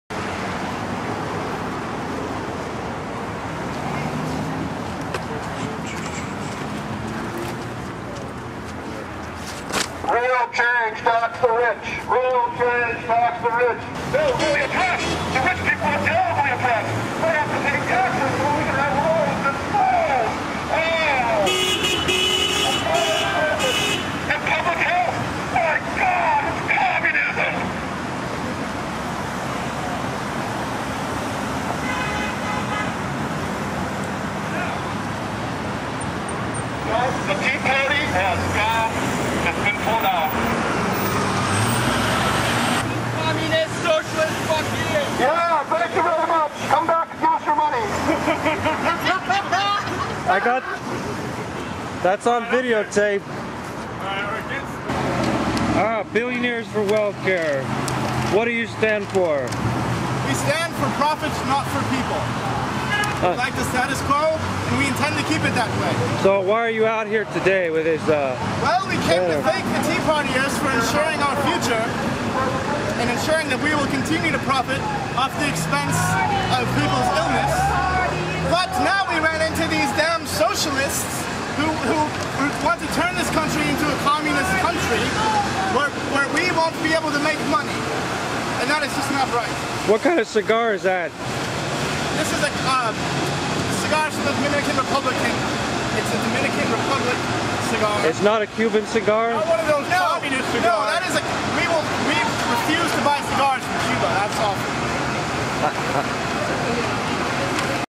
Anti-Tea Party Rally 4/15/2010
A group of activists comprising SDS - Students for a Democratic Society, The Peace and Freedom Party, and Green Party took to the streets to counterpose the reactionary and backwards Tea Party movement. While the reactionaries yelled at us "FU#*ING Communists, Socialists Go Home!!", we were there to stay. Progressive and Radical forces of the Left together in solidarity against the Right Wing, Conservative- Reactionary Establishment Tea Party movement. We were an exemplary contingent. FIGHT THE POWERS THAT BE!!